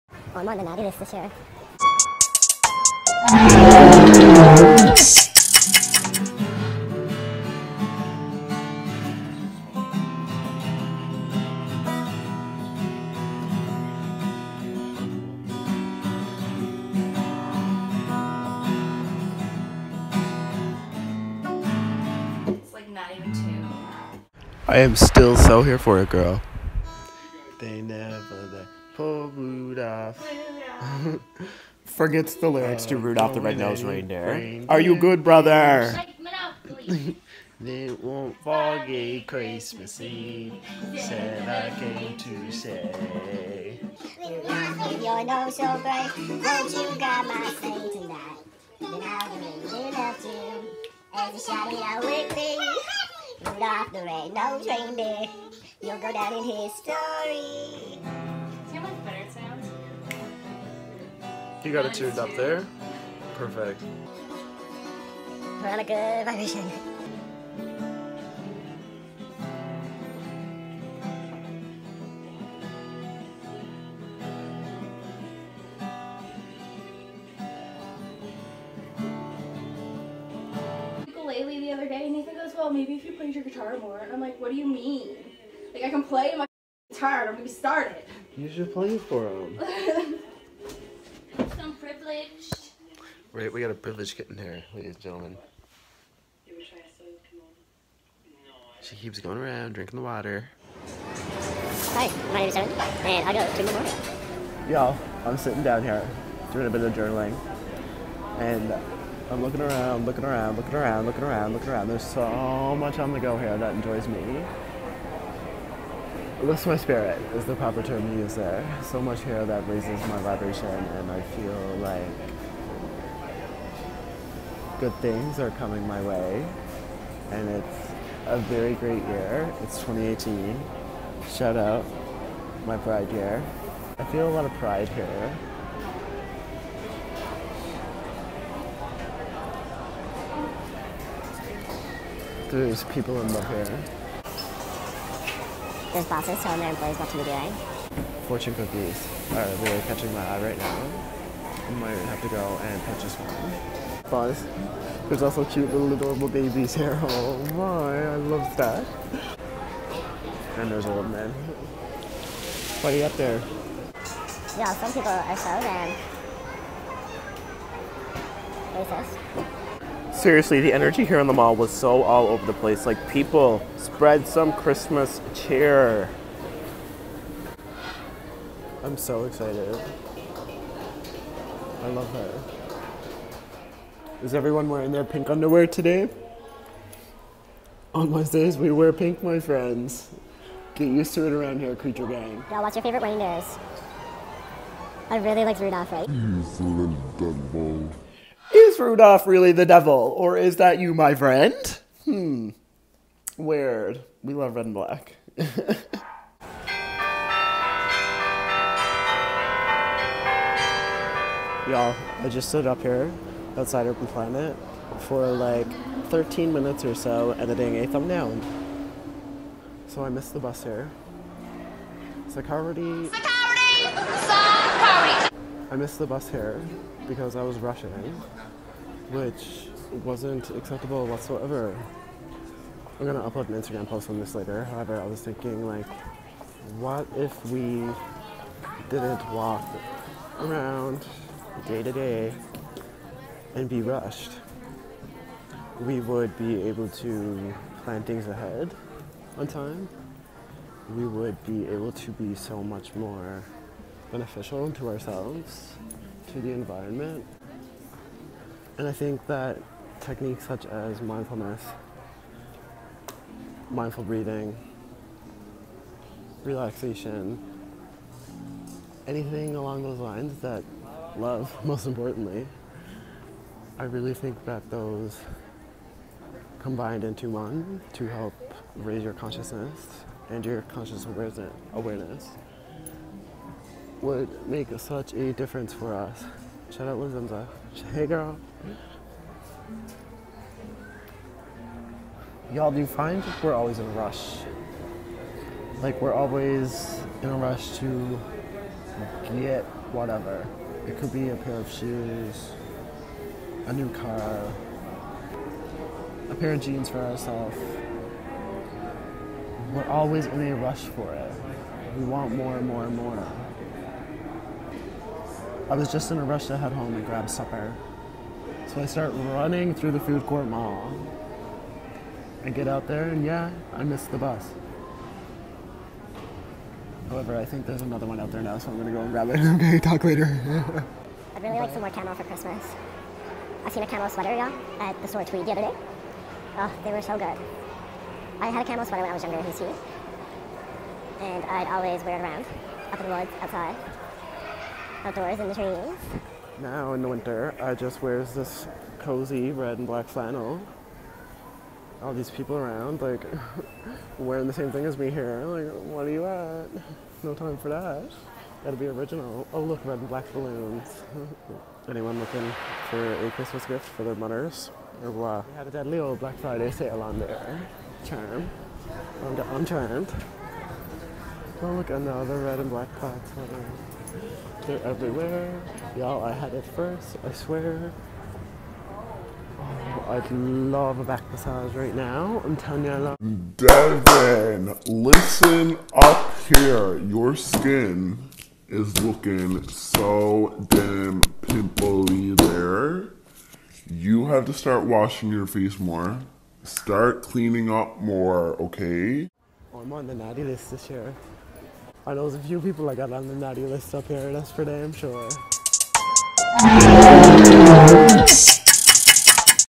There’s People In Love Here! | Movember
Hey creatures! How was your weekend!? Mine was absolutely filled with tons of Christmas joy, cheer, & of course there was tons of magic surrounding the air.

Today’s video is a DailyVlog mixed with some friends, guitar, & Christmas cheer. Vlogmas IS ALMOST HERE!